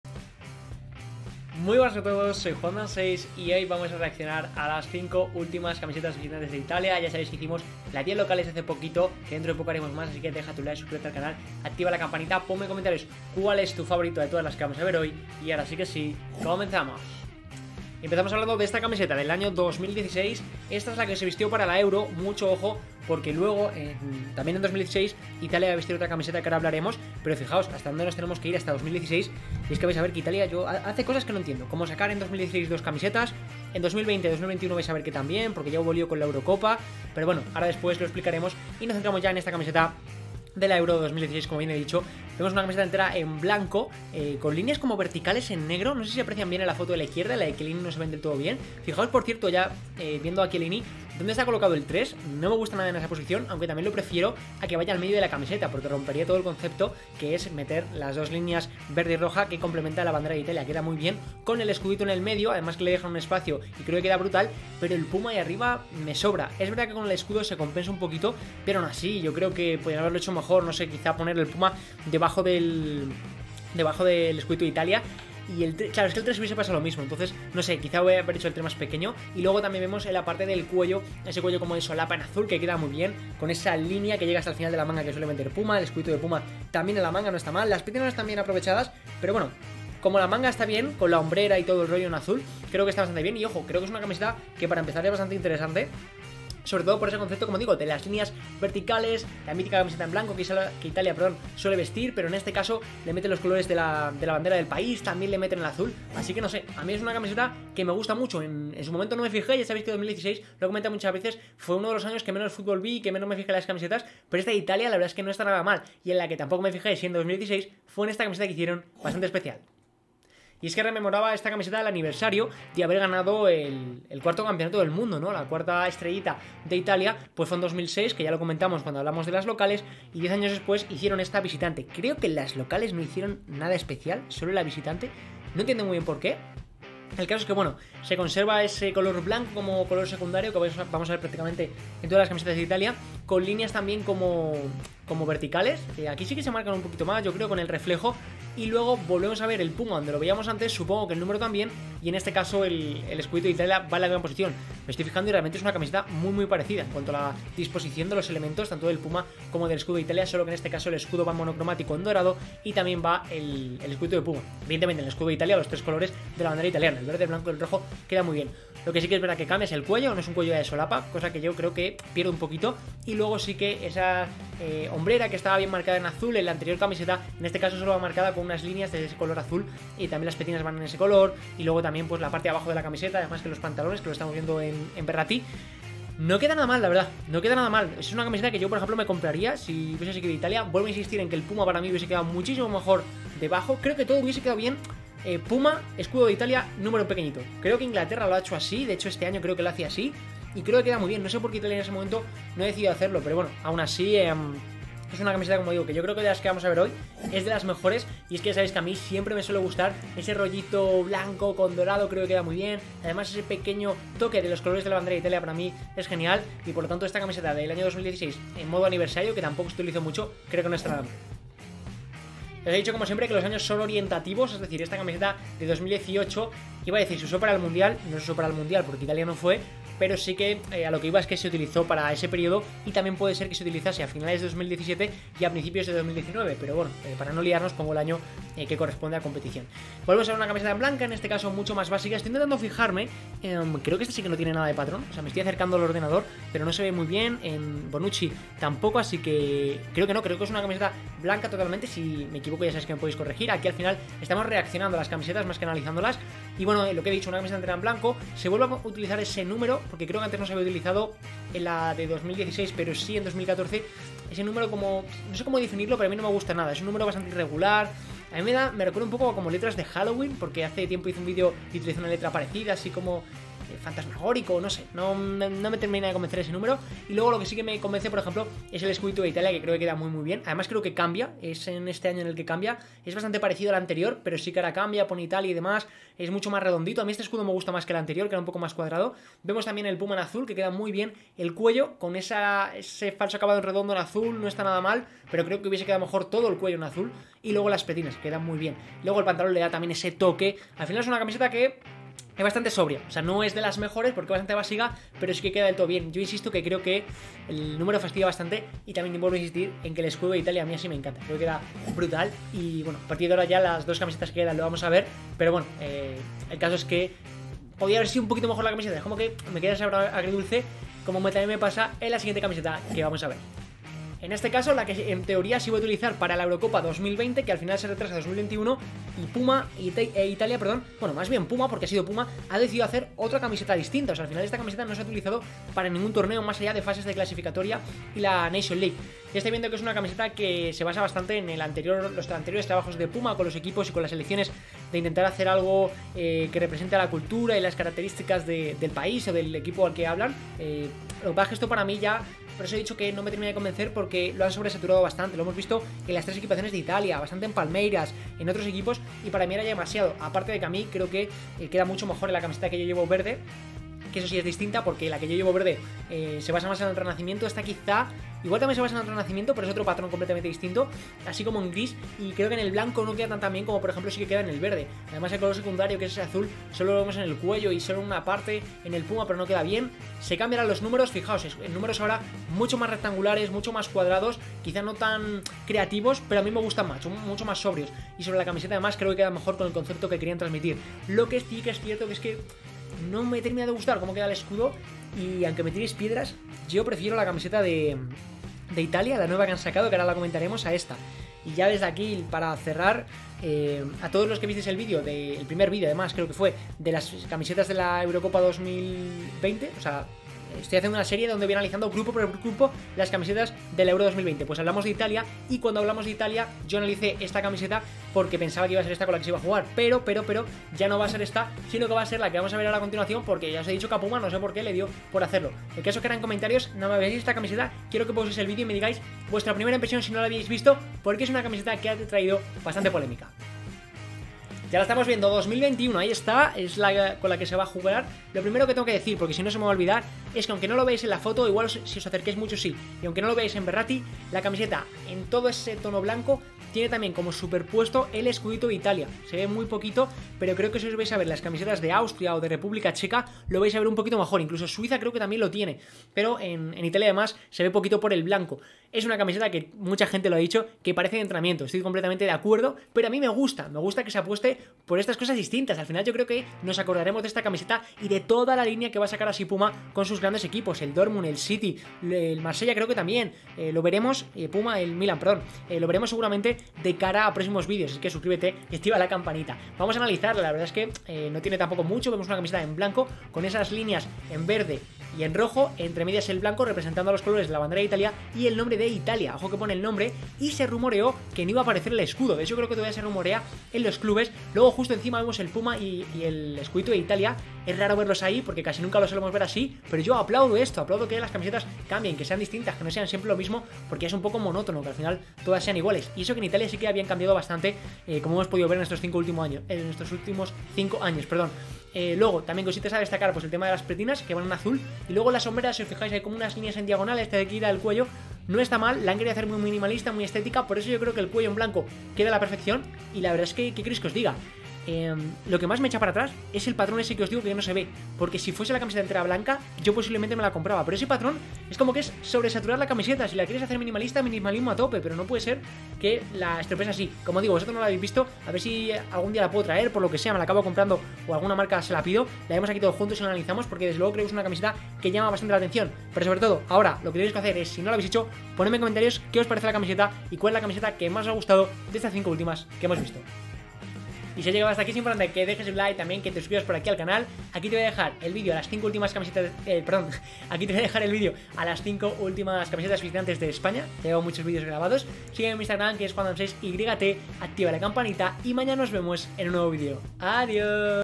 🇮🇹 REACCIONANDO a las 5 ÚLTIMAS CAMISETAS VISITANTES de ITALIA
Hoy reacciono a todas las camisetas visitantes de la seleccion italiana desde 2016